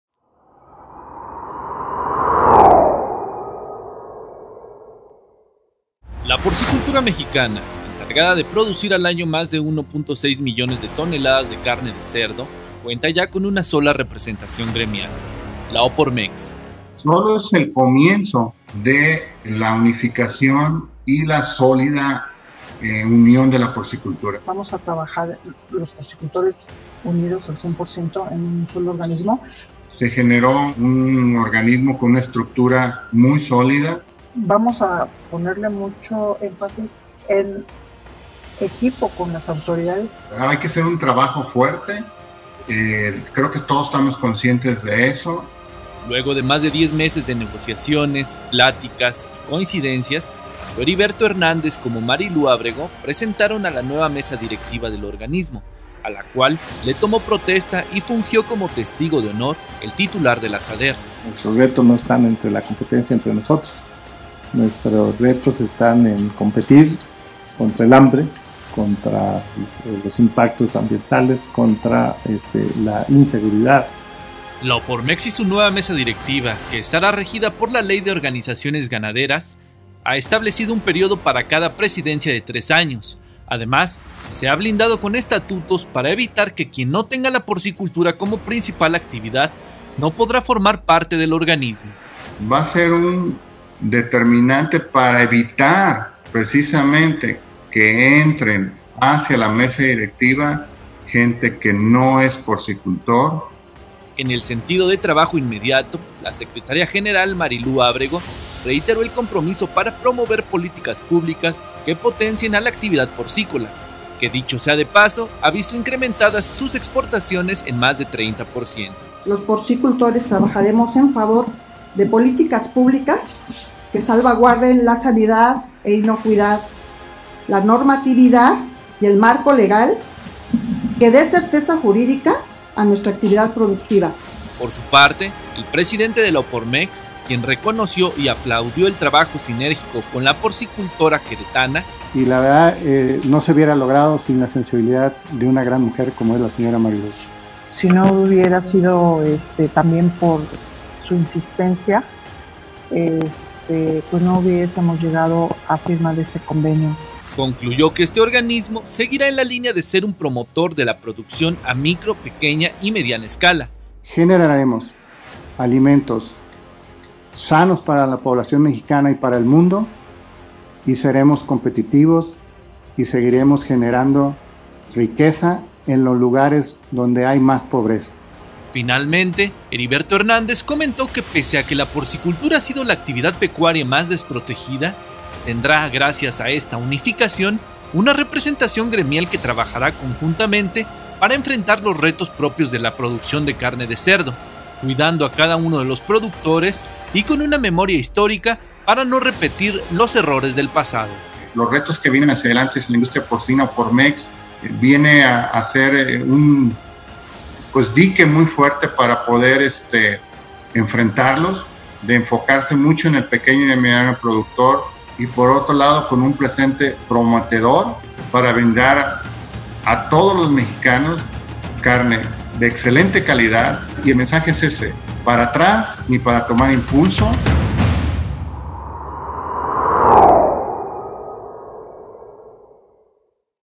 Arrancan los trabajos de la Opormex
La creación del Instituto Nacional del Cerdo, la capacitación virtual, el impulsar el cabildeo en Gobierno para que se apoye a los porcicultores de entre 10 y 300 vientres (los más desprotegidos); serán de las primeras acciones tangibles de la Opormex.

Platicamos en exclusiva con parte de la mesa directiva del nuevo organismo que representa a la porcicultura mexicana; Heriberto Hernández Cárdenas, presidente de la Organización de Porcicultores Mexicanos (Opormex) anticipó la creación del Instituto Nacional del Cerdo, mientras que Marilú Ábrego Chávez, secretaria general del organismo, recordó que las líneas de trabajo, políticas públicas, y necesidades de los productores estarán acompañadas en todo momento de las autoridades de gobierno.

Luego de que el titular de la Secretaría de Agricultura y Desarrollo Rural (Sader), Víctor Villalobos tomara protesta al presidente, a la secretaria general y al tesorero de la Opormex Manuel Santini Ibarra, —y en su primera aparición en medios de comunicación luego de este nombramiento—, Heriberto Hernández resaltó que dentro de los estatutos de la Organización se estableció como “candado” que quien no tenga a la porcicultura como actividad preponderante no podrá formar parte de la mesa directiva.